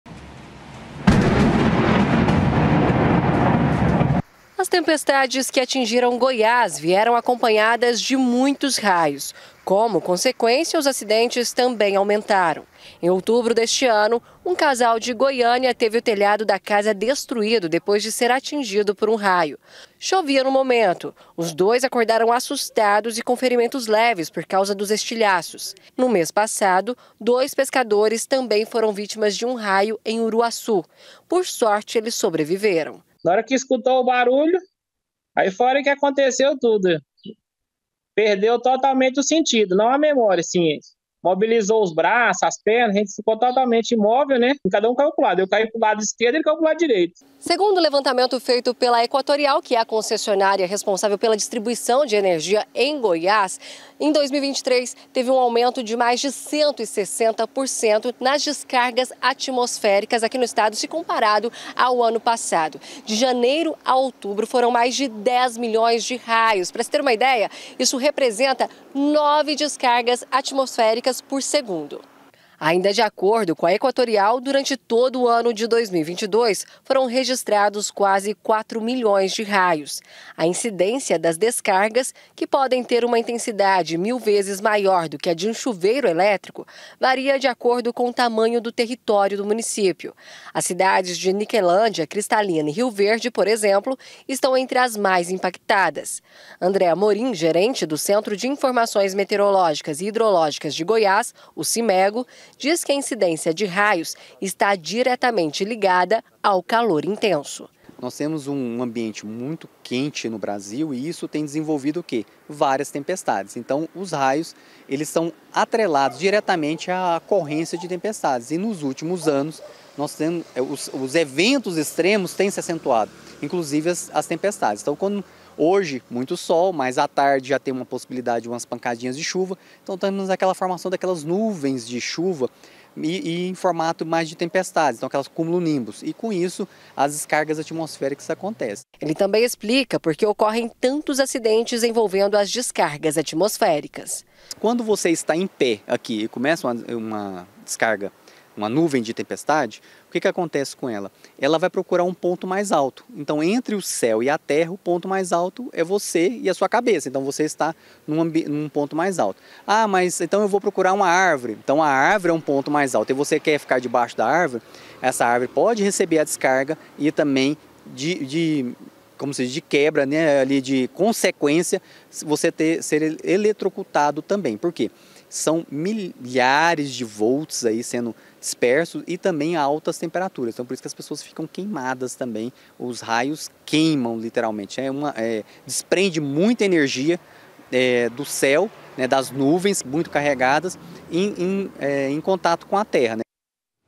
COMBINAÇÃO ELÉTRICA: FORTE CALOR PROVOCA AUMENTO NA OCORRÊNCIA DE RAIOS
O CALOR EXTREMO QUE TEM FEITO NOS ÚLTIMOS DIAS TRAZ UMA OUTRA CONSEQUÊNCIA: O AUMENTO DA INCIDÊNCIA DE RAIOS NESSE ANO, ATÉ AGORA, GOIÁS REGISTROU UM AUMENTO DE MAIS 160% NA OCORRÊNCIA DESSAS DESCARGAS ELÉTRICAS EM RELAÇÃO  A TODO O ANO PASSADO VOCÊ VAI ENTENDER AGORA A RELAÇÃO ENTRE O CALORÃO E O AUMENTO DOS RAIOS.